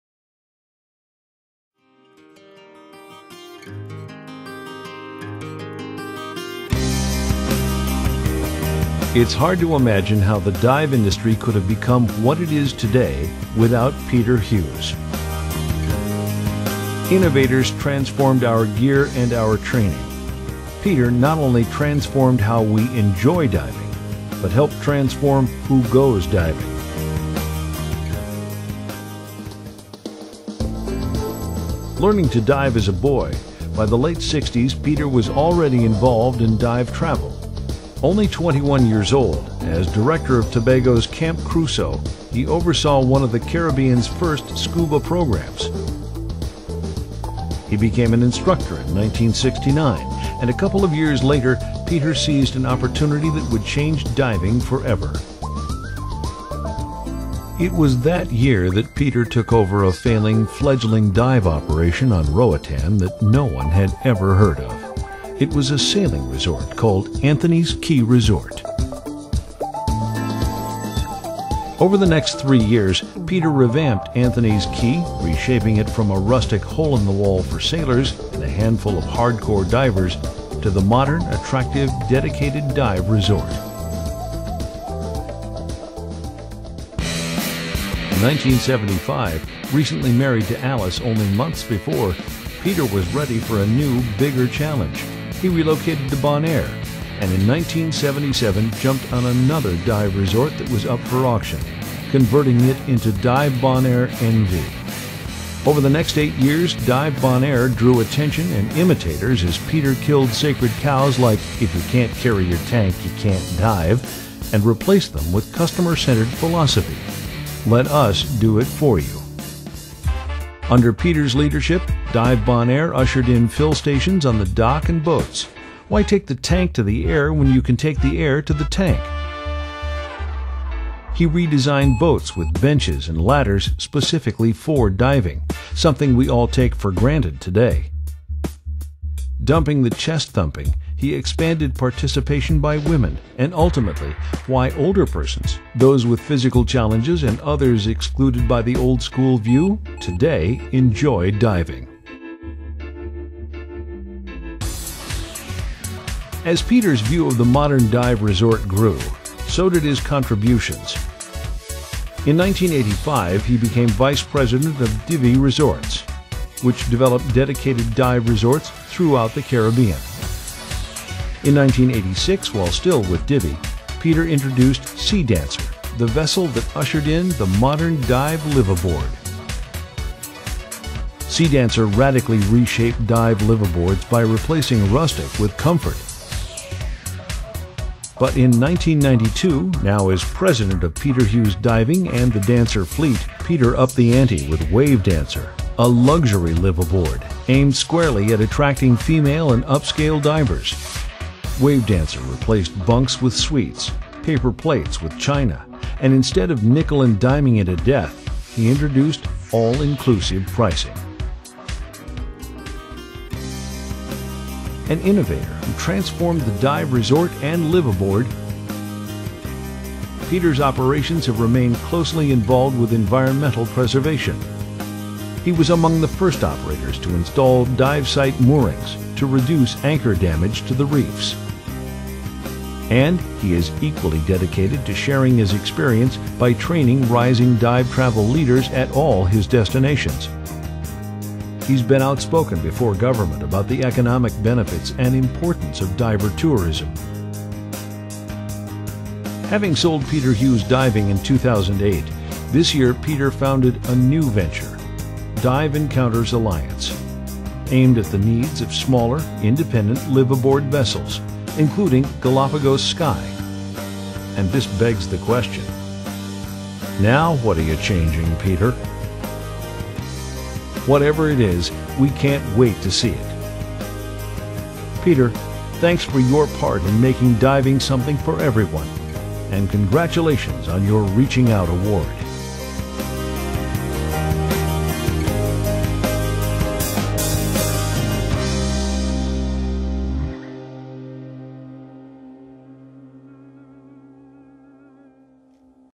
Peter Hughes, 2011 DEMA Reaching Out Award Recipient
First awarded in 1989, DEMA's Reaching Out Award recognizes those individuals that have had a positive impact on recreational diving by reaching out to help grow the industry.  Recipients are inducted into DEMA's Hall of Fame.

Past recipients of the Reaching Out Award include diving pioneers who have helped develop techniques related to their special areas of expertise, including science, equipment design, training, retailing, travel and safety.  To become a member of DEMA's Hall of Fame is an extraordinary achievement available only to those who truly deserve to be praised for their outstanding efforts and contribution to the sport of diving.  Past recipients of the ROA include Jacques-Yves Cousteau (1989), Dr. Sylvia Earle (1991), Zale Parry (1993), Lloyd Bridges (1994), Howard Hall (1999), David Doubilet (2007) and most recently Fraser Bathgate and Peter Hughes (2011).